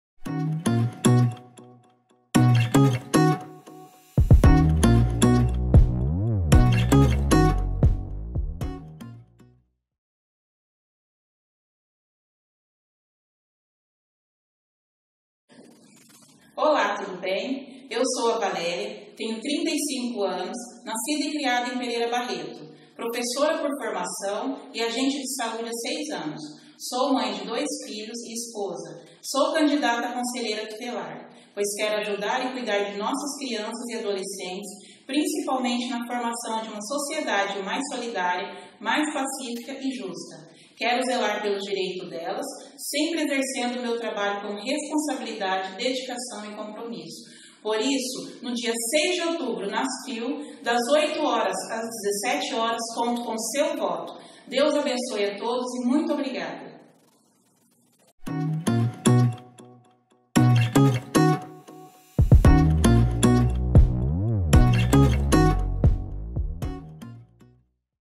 VALÉRIA FELIPE - Conheça os Candidatos a Conselheiro Tutelar de Pereira Barreto/SP 2019
Candidato Valéria Felipe - Pedagogia

Série de vídeos dos candidatos ao Conselho Tutelar de Pereira Barreto, idealizada para que a população em geral possa conhecer um pouco melhor os candidatos, a votação será realizada no próximo domingo (06/10) nas Faculdades Integradas Urubupungá (Av: Jonas Alves de Mello, 1660) das 8h às 17h.

SUPERVISÃO
Secretaria Municipal de Assistência Social

CAPTAÇÃO, PRODUÇÃO E EDIÇÃO
Setor de Tecnologia da Informação

LIBRAS
VLIBRAS – vlibras.gov.br

Compareça ao local de votação com Título de eleitor e um documento de identificação oficial com foto ex:  (RG ou CNH).

As Eleições vão ocorrer no dia 06/10 das 8h às 17h nas Faculdades Integradas Urubupungá de Pereira Barreto, na Av: Jonas Alves de Mello,1660